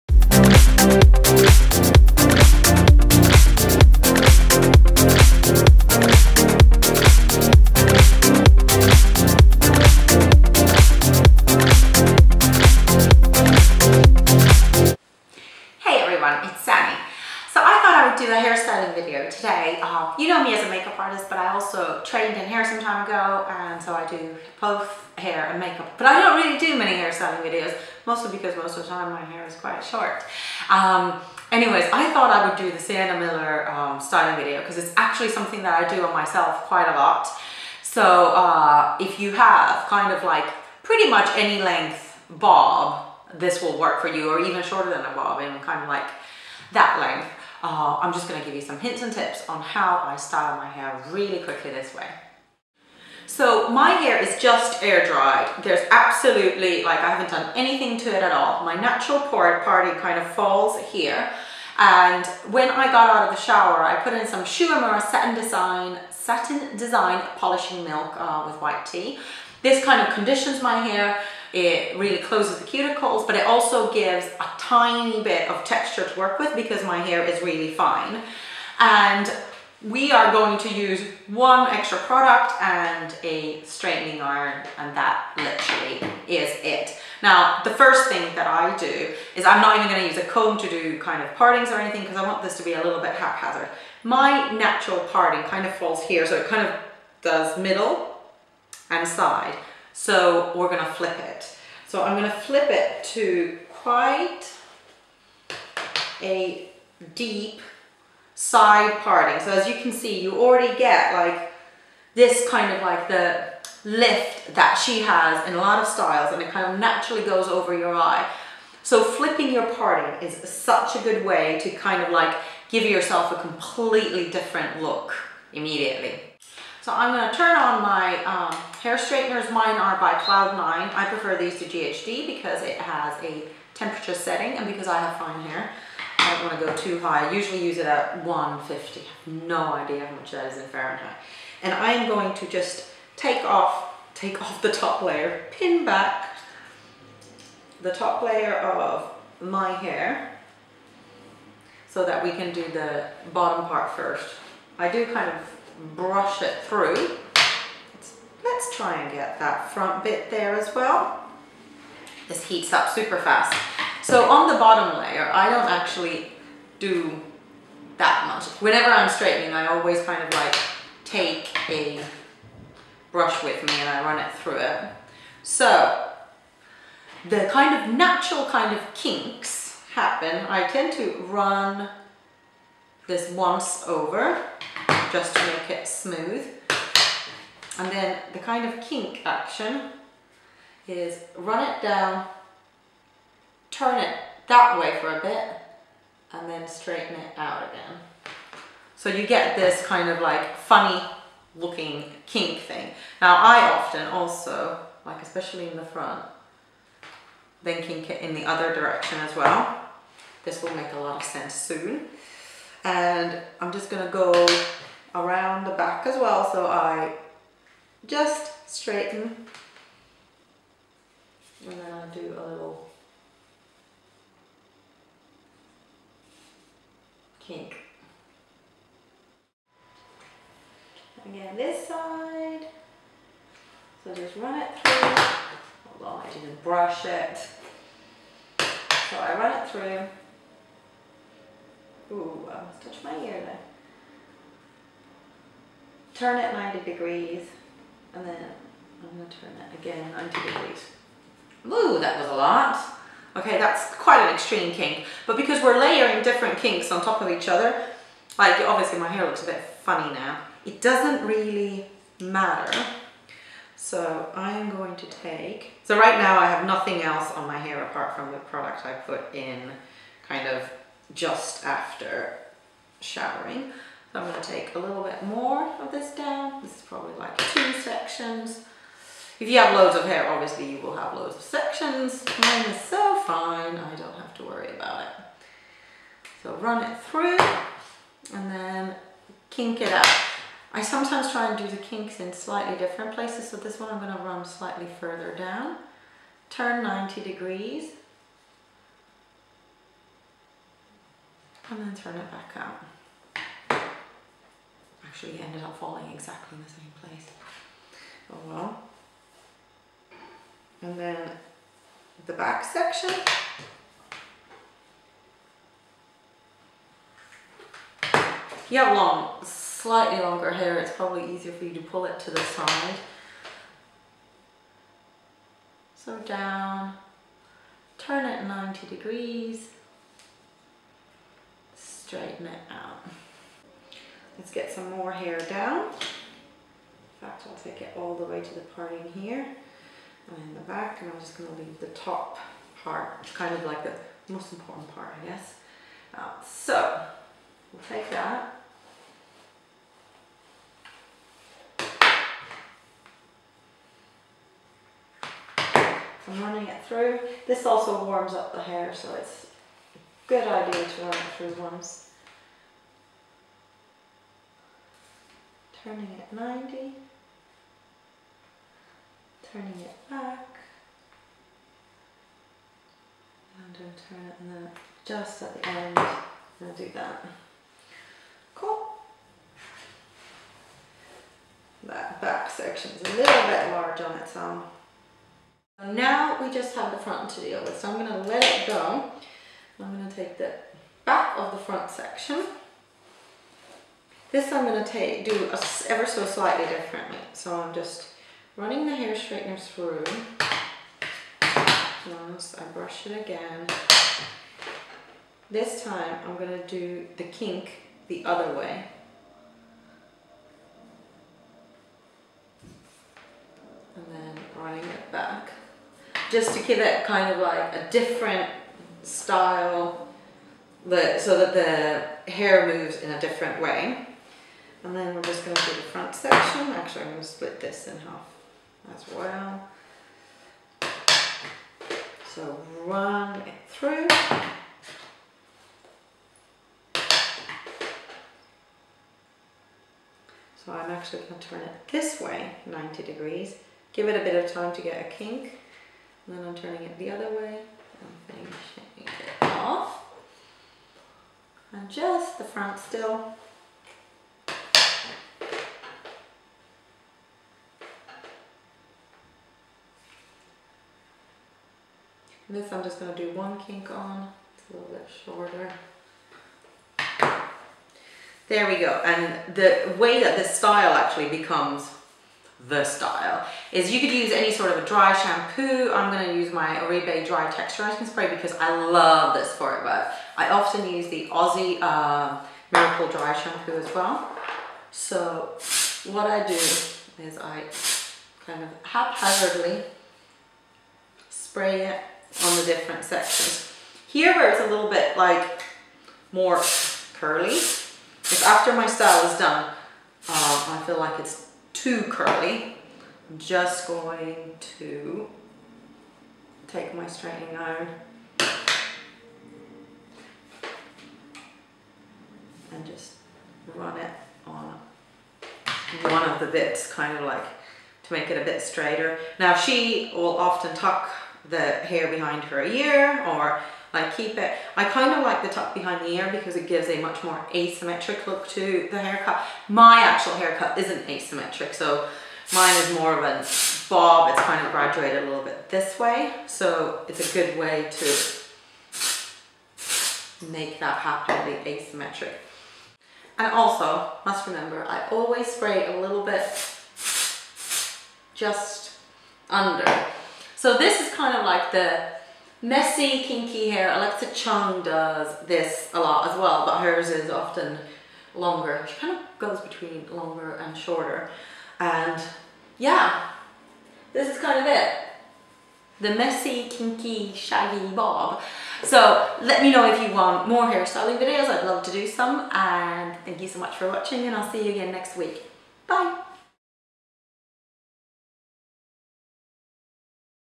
How to style Sienna's shaggy bob
❥❥ Expand with 'Show more' below for all the products and info

This is how I style my hair when I want to do the shaggy Sienna bob  : ) Its super easy, you just need to turn the straightening iron 90 degree's and you're there, boom! Lots of love and let me know if you want more hairstyling videos, Sanni xo